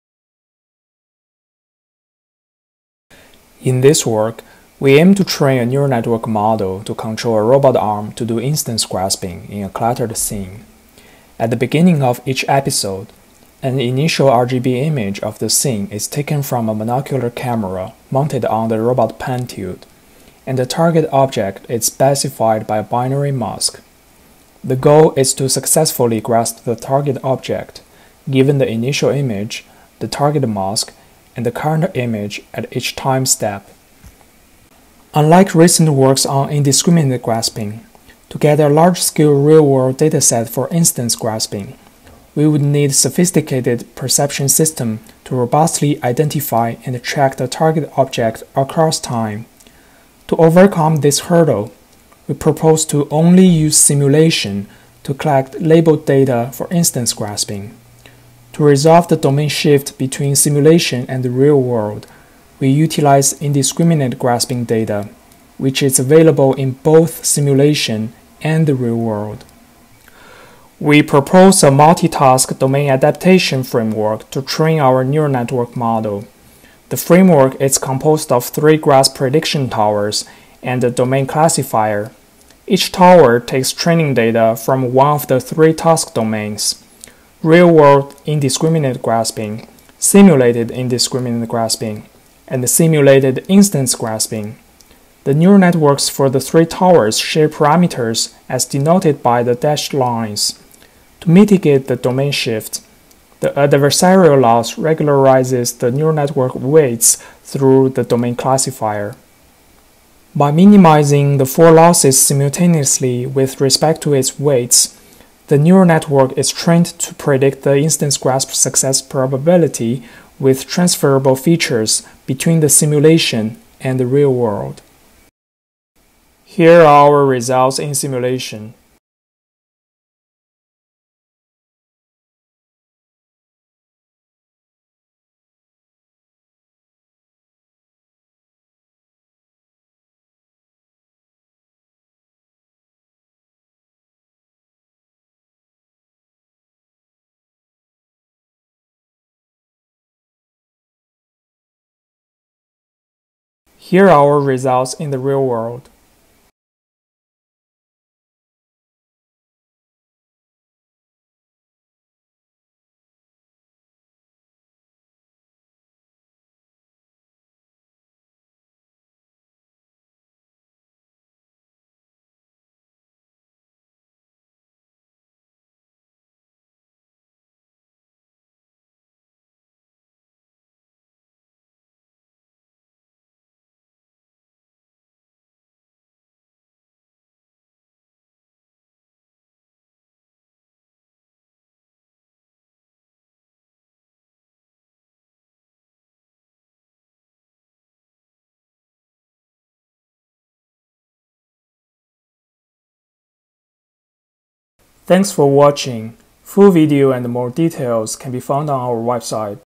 Multi-Task Domain Adaptation for Deep Learning of Instance Grasping from Simulation
Learning-based approaches to robotic manipulation are limited by the scalability of data collection and accessibility of labels. In this work, we present a multi-task domain adaptation framework for instance grasping in cluttered scenes by utilizing simulated robot experiments. Our neural network takes monocular RGB images and the instance segmentation mask of a specified target object as inputs, and predicts the probability of successfully grasping the specified object for each candidate motor command. The proposed transfer learning framework trains a model for instance grasping in simulation and uses a domain-adversarial loss to transfer the trained model to real robots using indiscriminate grasping data, which is available both in simulation and the real world. We evaluate our model in real-world robot experiments, comparing it with alternative model architectures as well as an indiscriminate grasping baseline.